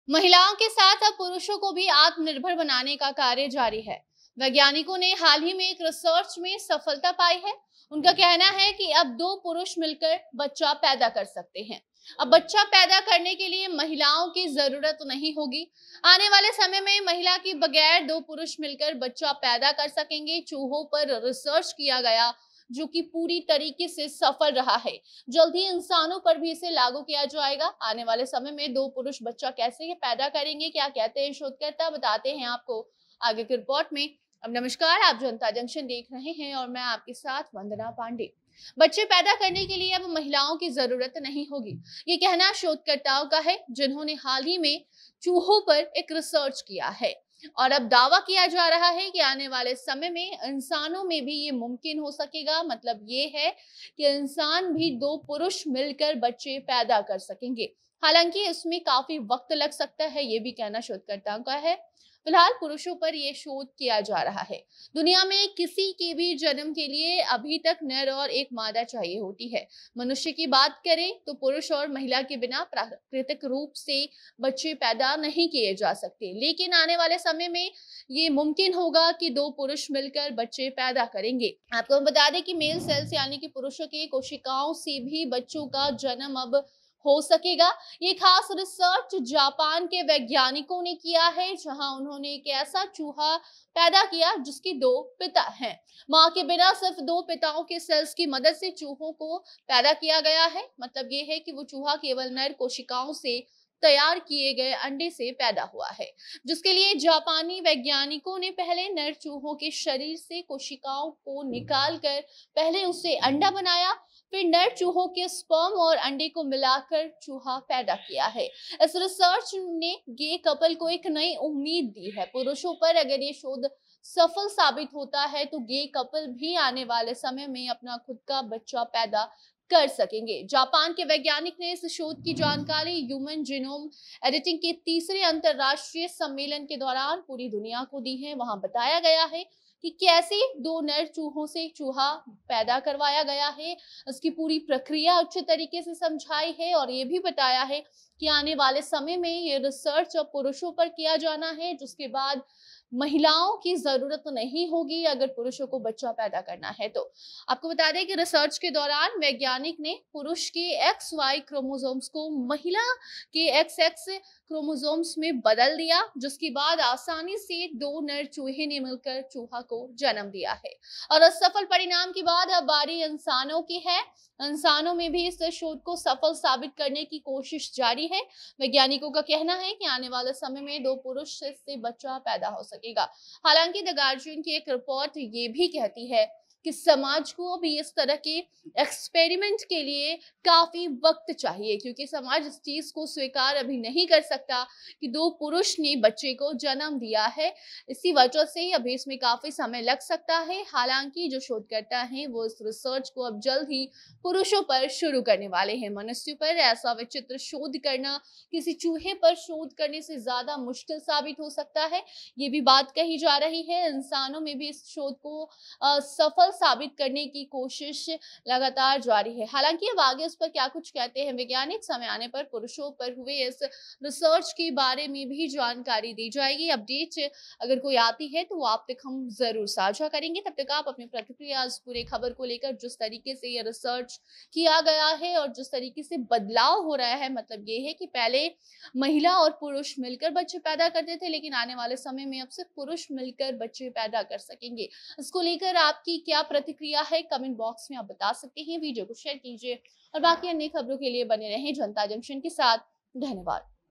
महिला के बगैर पुरुष बच्चों को जन्म देंगे,चूहों के बाद अब पुरुषों पर होगा रिसर्च!जानें पूरी रिपोर्ट!
"पापा प्रेगनेंट हैं" कहेंगे बच्चे,भविष्य में महिलाओं के बिना दो पुरुष मिलकर बच्चे पैदा कर पाएंगे!जापान के शोधकर्ता चूहों के बाद अब पुरुषों पर रिसर्च करेंगे,देखिए पूरी रिपोर्ट!